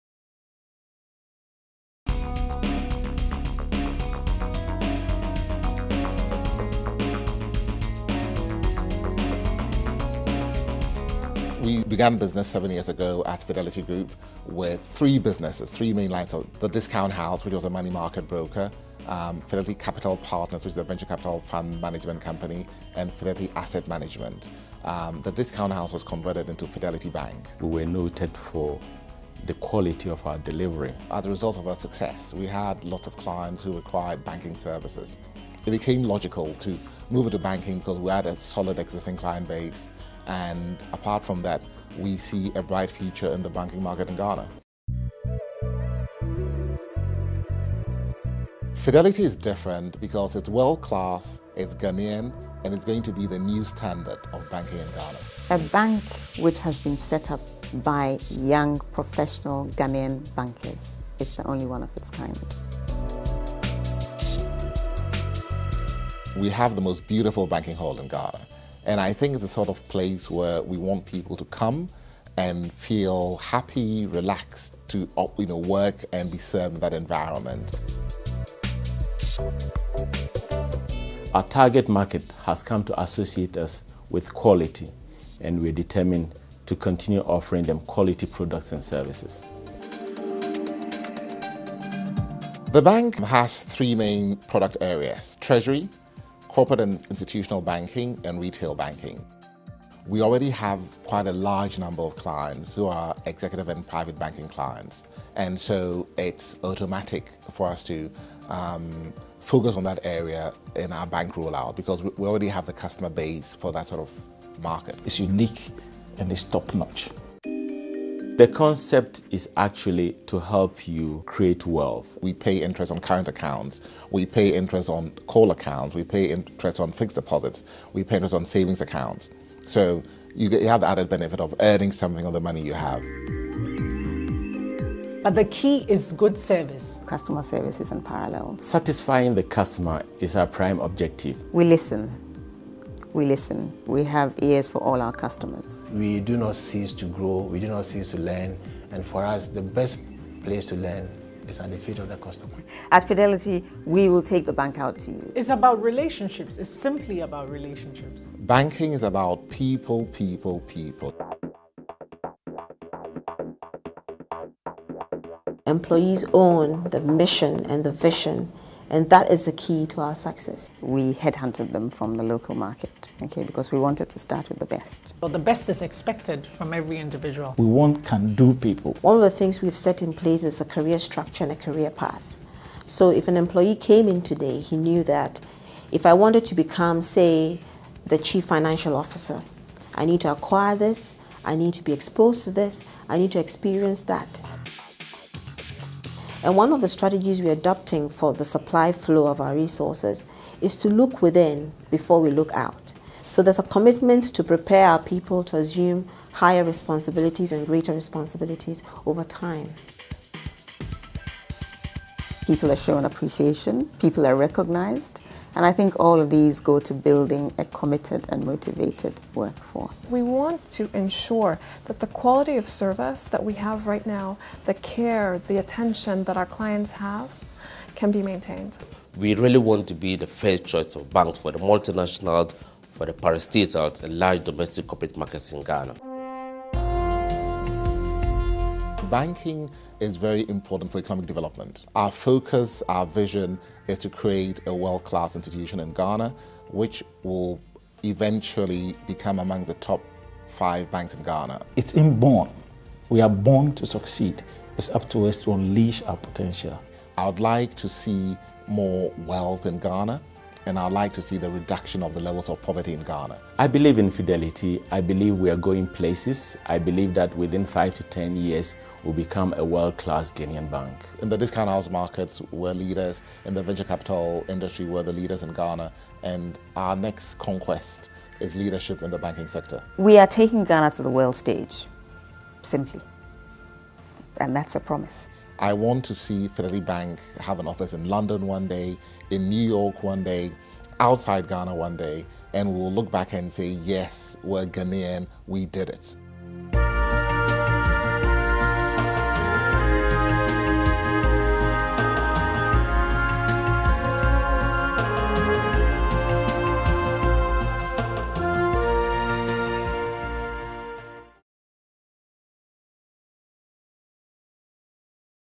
Fidelity Bank Documentary
Documentary of Fidelity Bank Ghana.